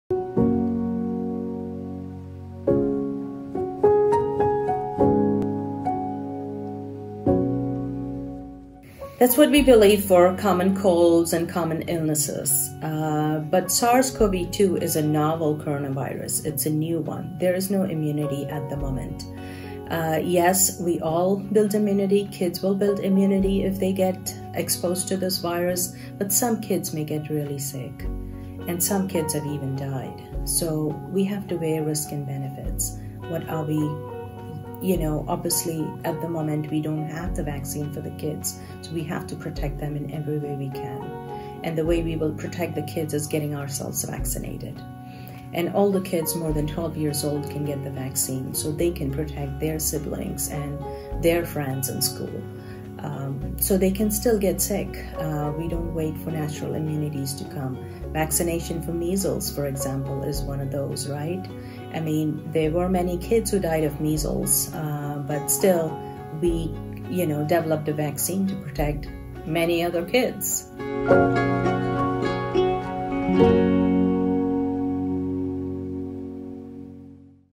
Vaccine Fact Finding: Can children build immunity
Unvaccinated people are more susceptible to the virus and its variants, and people who are not vaccinated make up most COVID-19 hospitalizations today. Many individuals could avoid the hospital entirely if they had been vaccinated.

Still, there are many eligible individuals who have not been vaccinated, citing a number of reasons or concerns. In this episode, we bring those concerns to infectious disease expert Aneesa Afroze, MD, to provide additional context and dispel common myths.